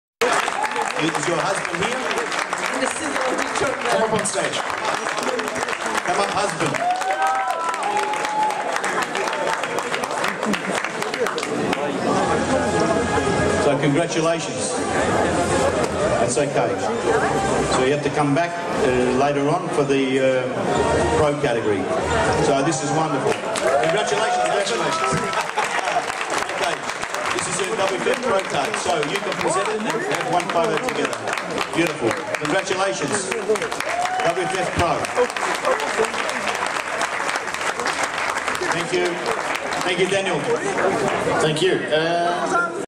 WFF Universe 2015 - Women ExtremeBody - Karolína Borkovcová (receiving Pro Card)
2015/06/27 at La Ciotat, France. Women ExtremeBody Category.
#200 Karolína Borkovcová (Czech Republic)
Originally filmed with Olympus OM-D EM-5 @ 1280x720px.